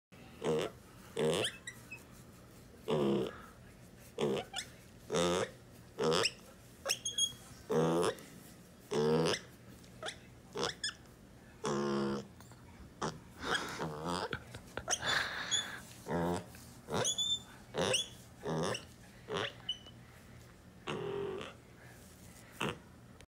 Happy pig
When your pig is happy to get love!